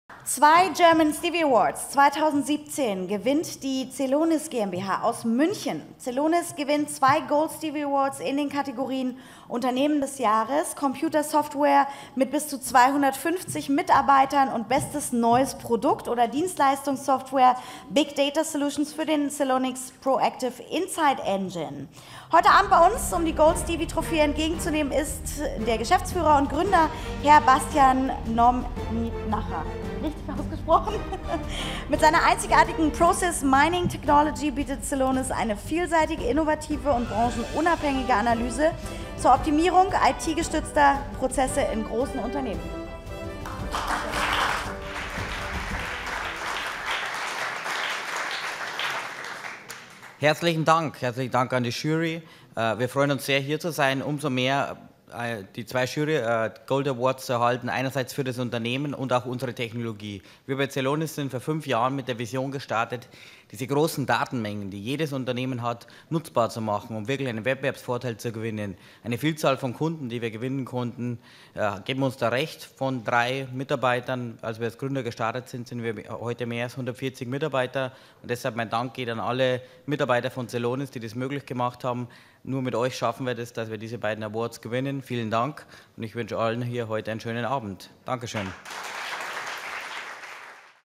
Celonis, GmbH gewinnt bei den German Stevie Awards 2017
Die German Stevie Awards sind ein hochkarätiger Wirtschaftspreis für die deutsche Unternehmenswelt.

Die German Stevie Awards 2017 wurden am 31. March 2017 verliehen im Hamburger Fairmont Hotel Vier Jahreszeiten. Hochrangige Vertreter von über 50 Gewinnerunternehmen aus ganz Deutschland nahmen ihre Auszeichnungen auf der Bühne in Empfang.

Die Bewerbungen für die German Stevie Awards 2018 können Sie ab August 2017 einreichen.

The German Stevie Awards are a high-caliber economic award for the German business world.

The German Stevie Awards 2017 were presented on March 31, 2017 at the Hamburg Fairmont Hotel Vier Jahreszeiten. High-ranking representatives from more than 50 winners from all over Germany received their awards on stage.

You can submit the applications for the German Stevie Awards 2018 from August 2017 onwards.